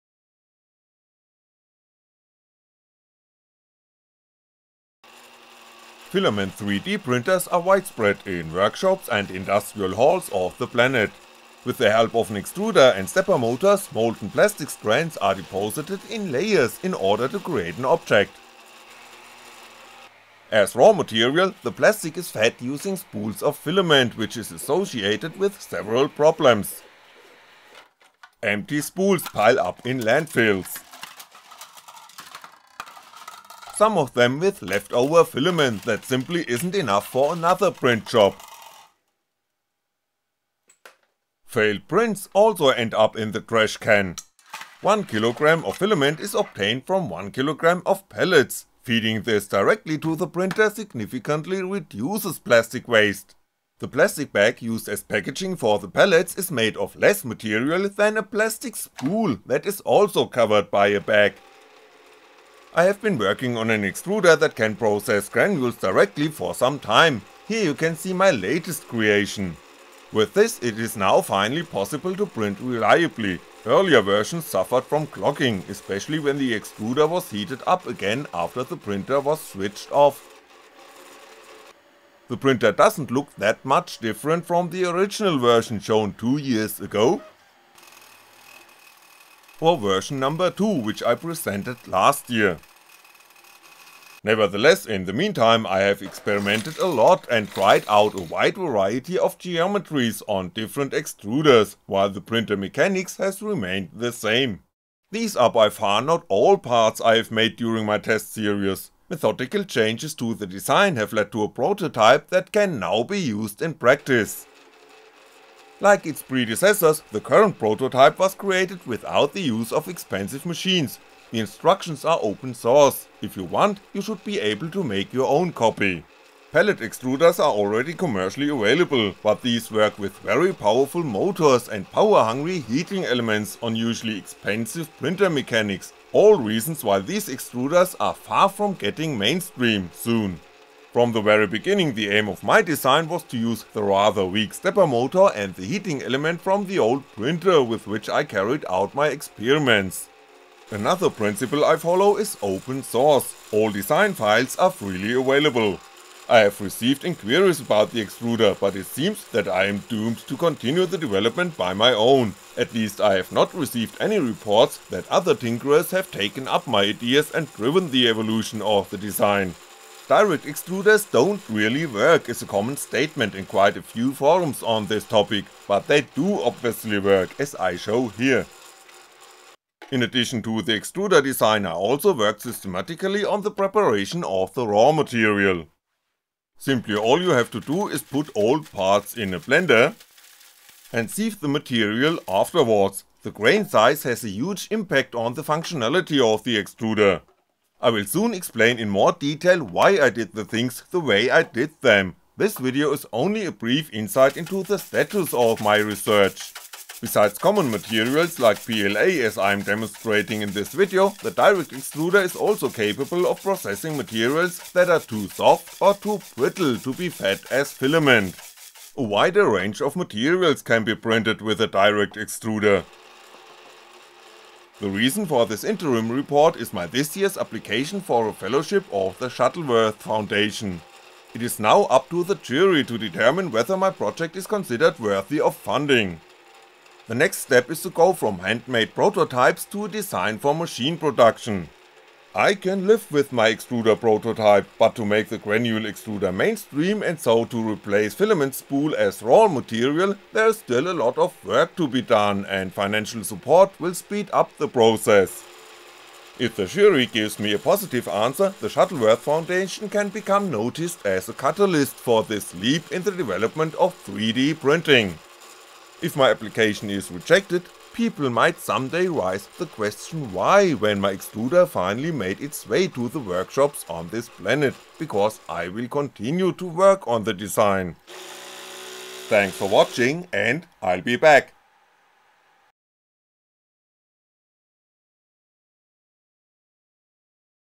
Application for a fellowship of the Shuttleworth Foundation in 2022
Table Of Content:
00:00 Introduction
00:19 Spools versus pellets
01:21 Extruder evolution
01:54 Current version
02:58 Preparing raw material
03:25 Conclusion